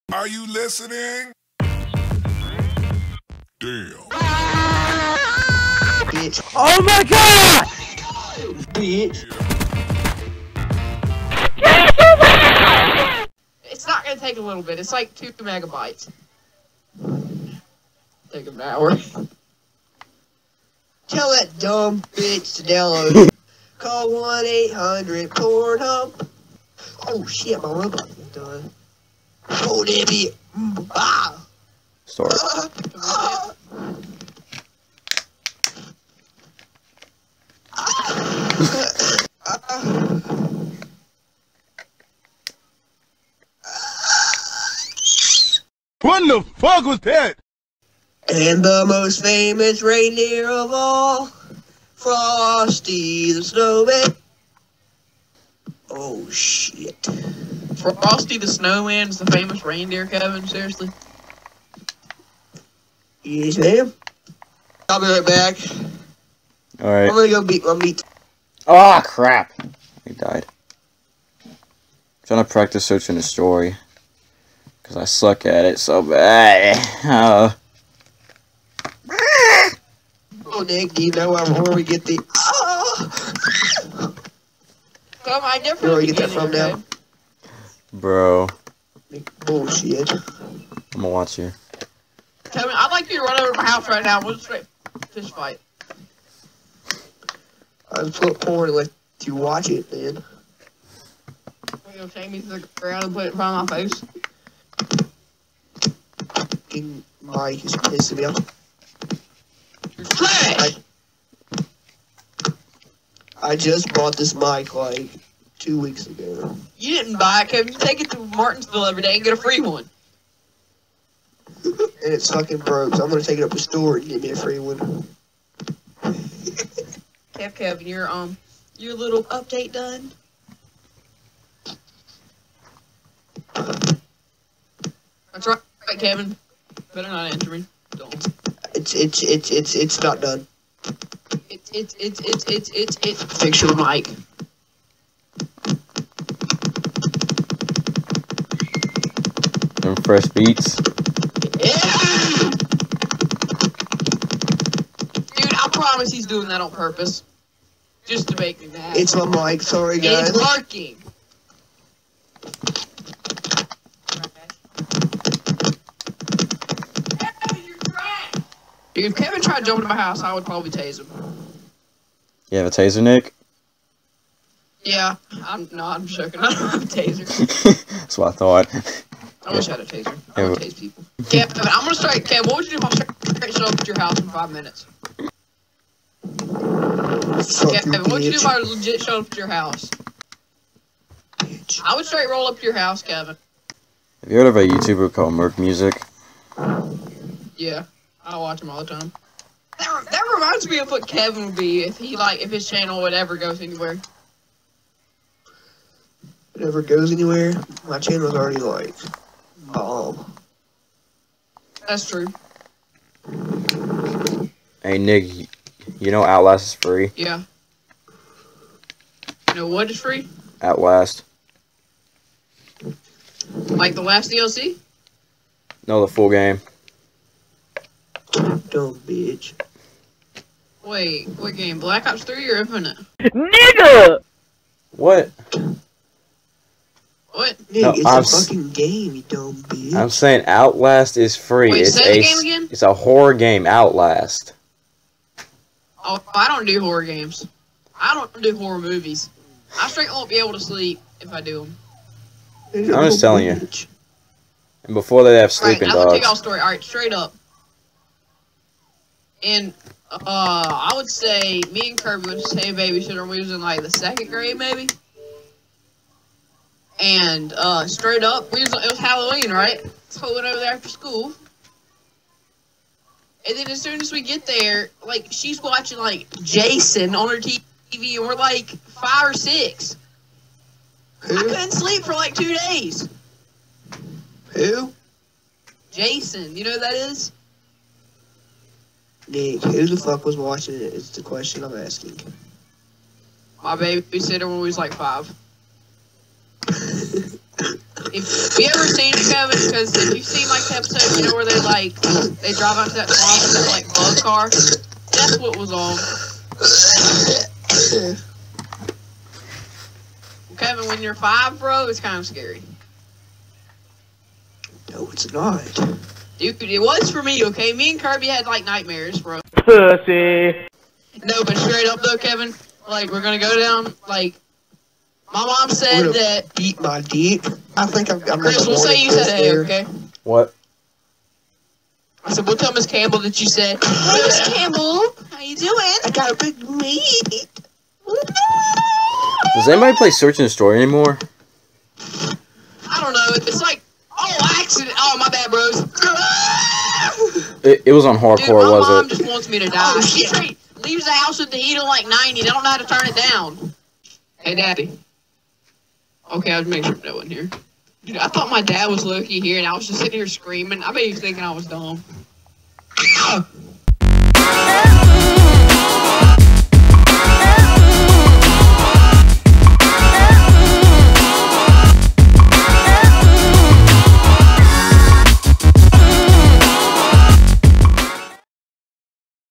Xbox Party Chat #11
Me, FUSION, and FRENZY in another party chat. Hope you enjoy the video